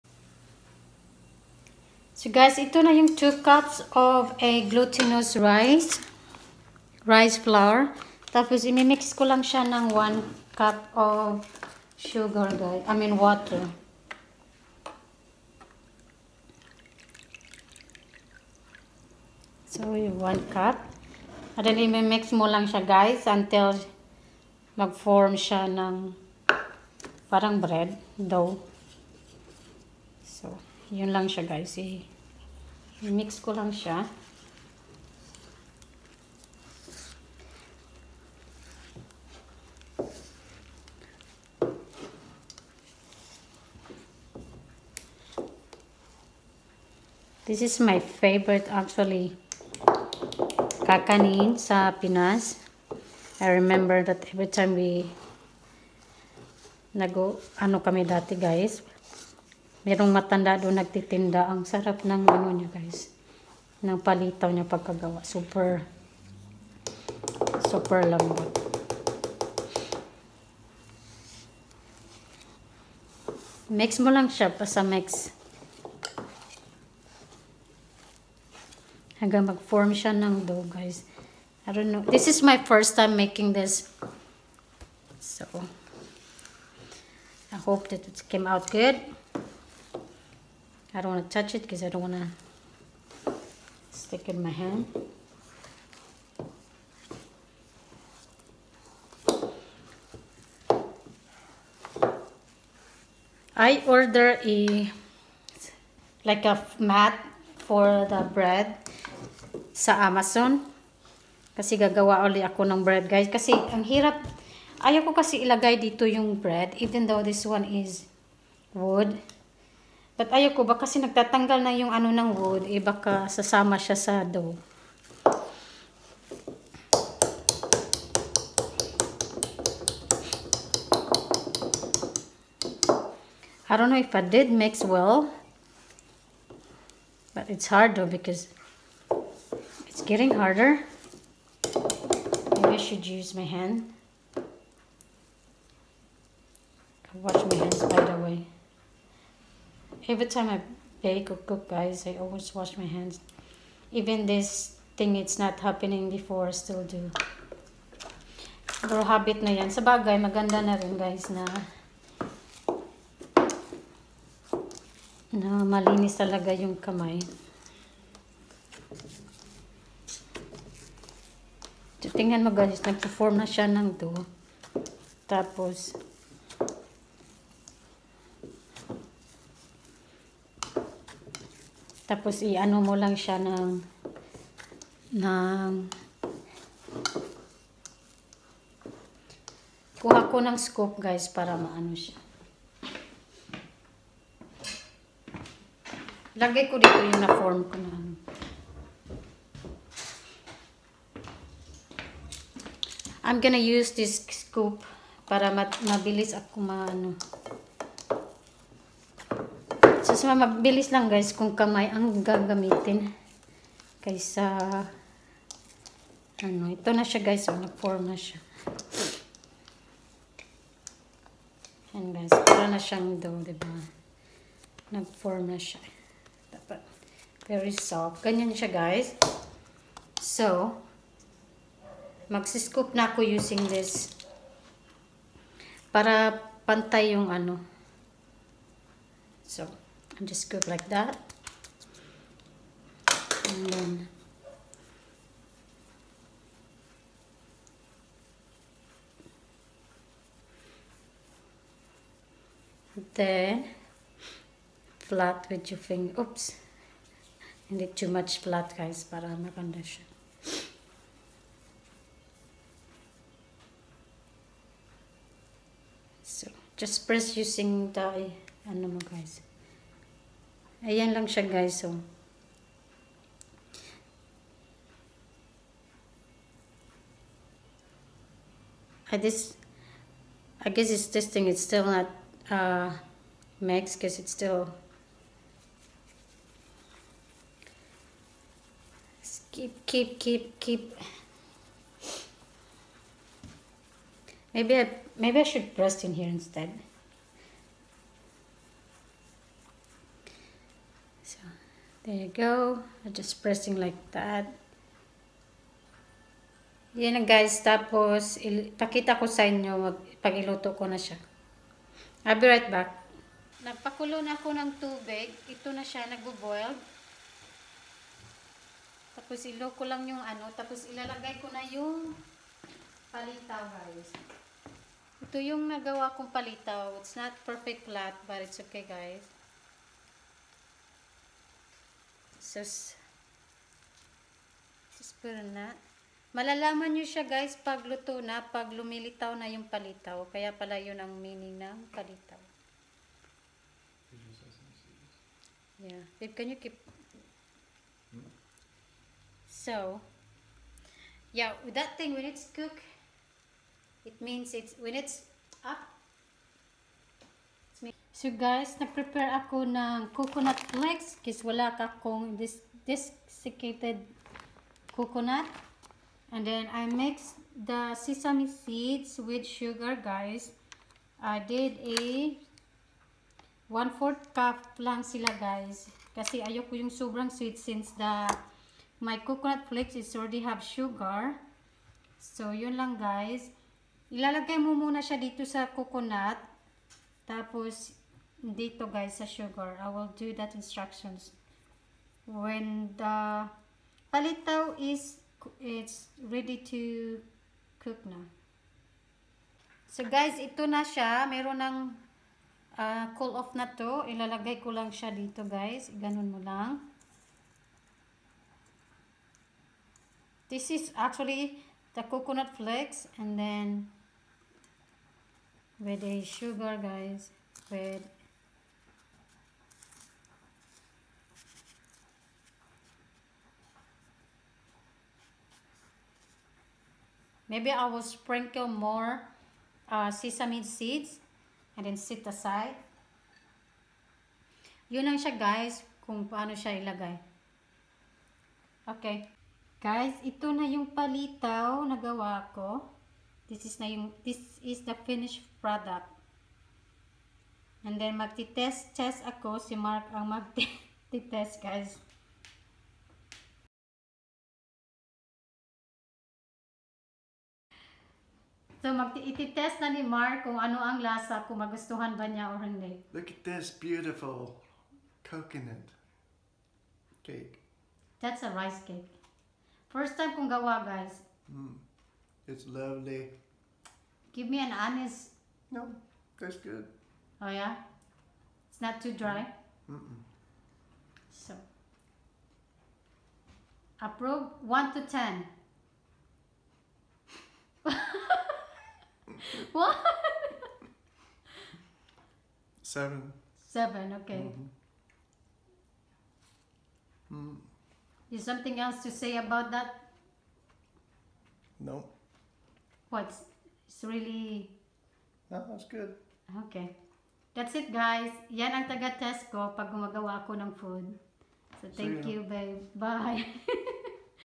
First Time Making Palitaw from Scratch
guys gumawa ako ng palitaw from scratch easy lang pala siya gawin.